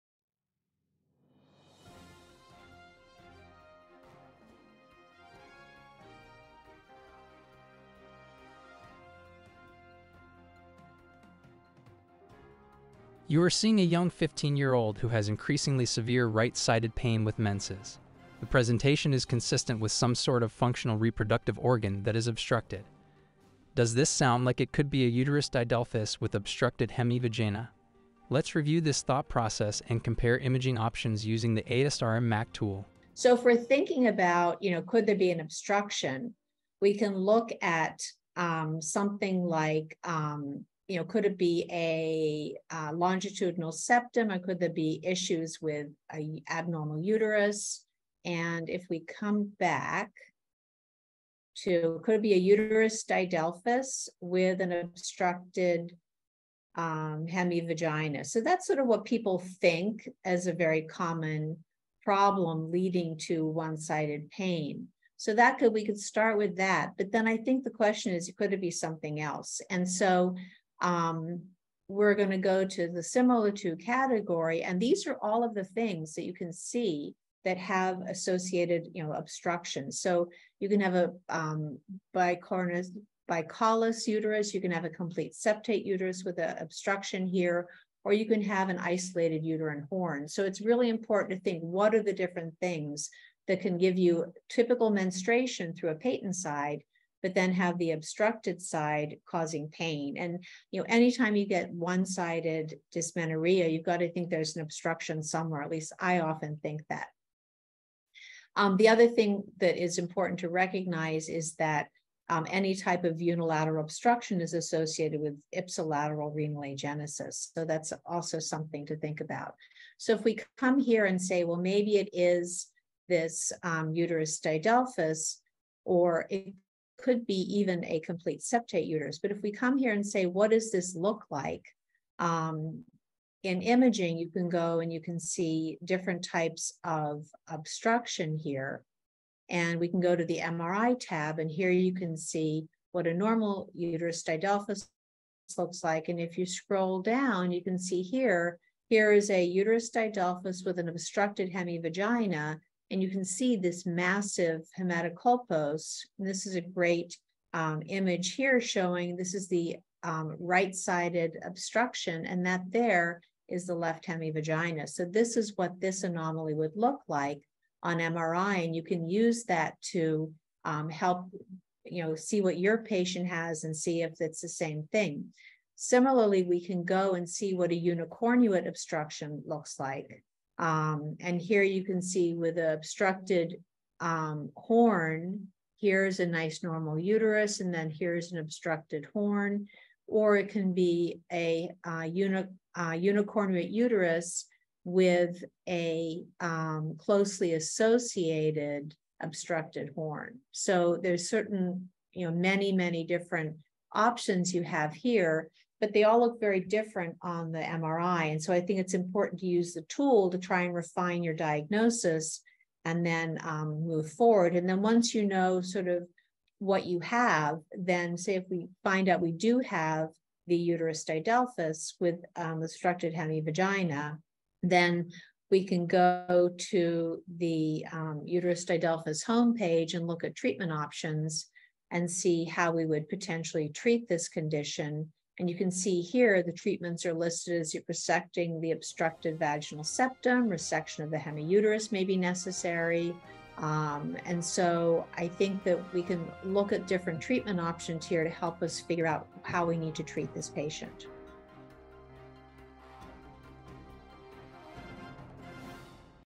ASRMed Talk: Interactive MAC Tool
View the Interactive tool for classifying mullerian anomalies